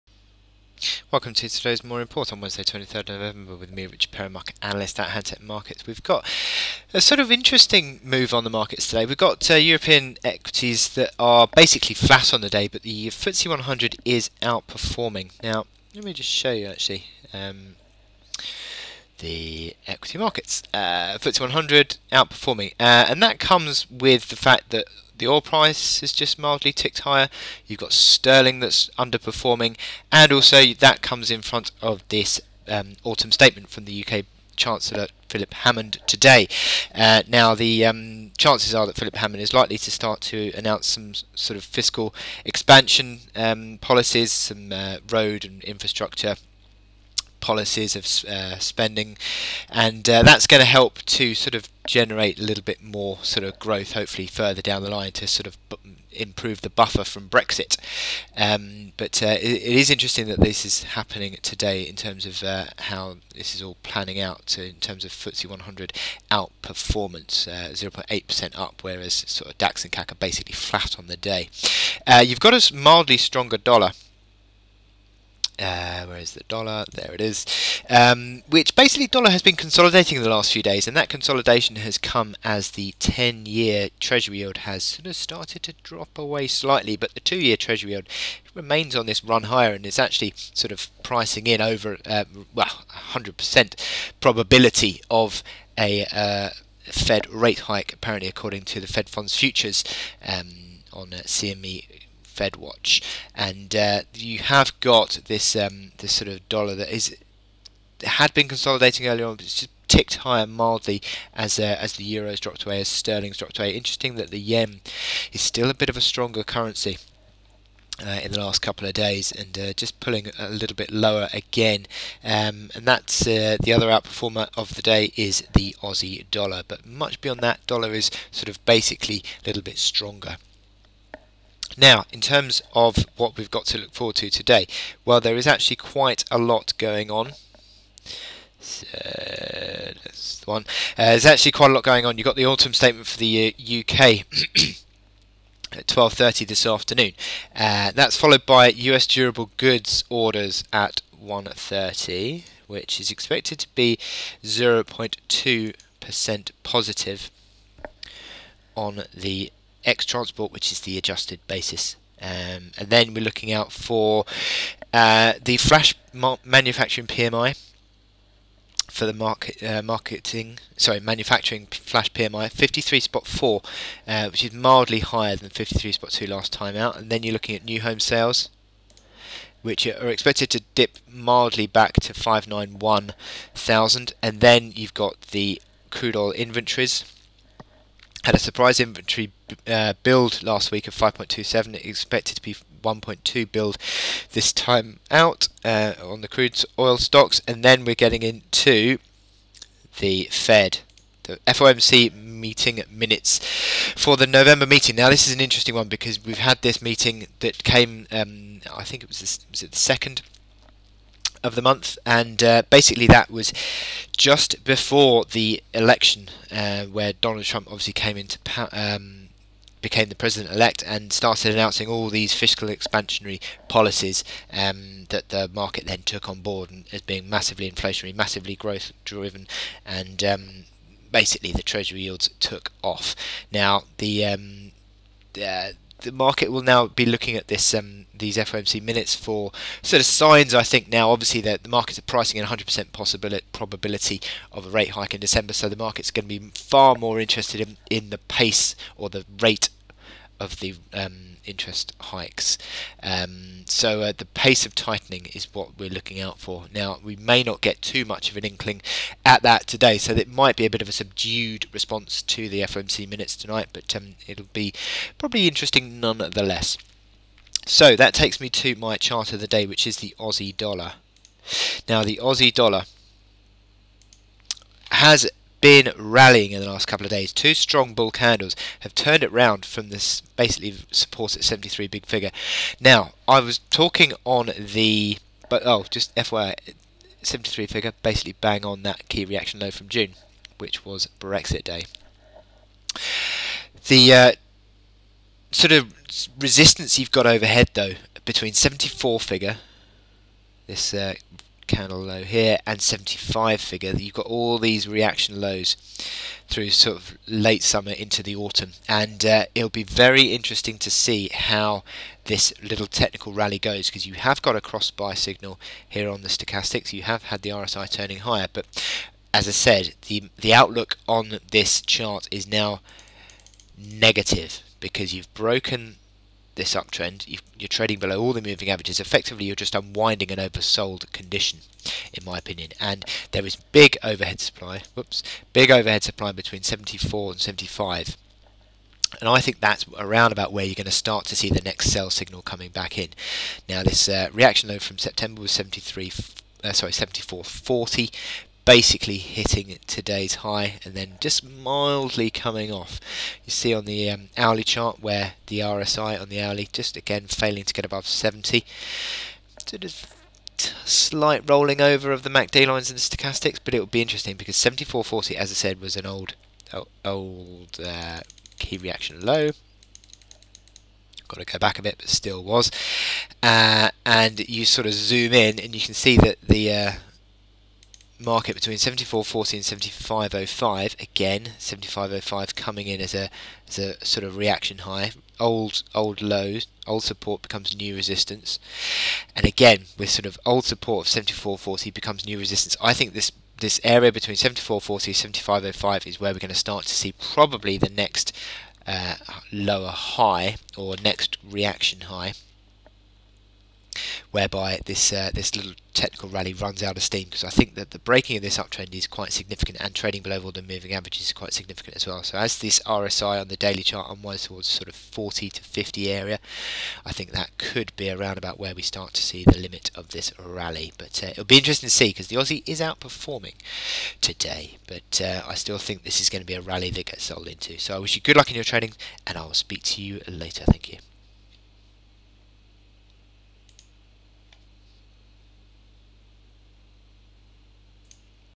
Daily Market Overview 23/11/2016
A look at th drivers of market moves today and what to look out for. The chart of the day is Aussie dollar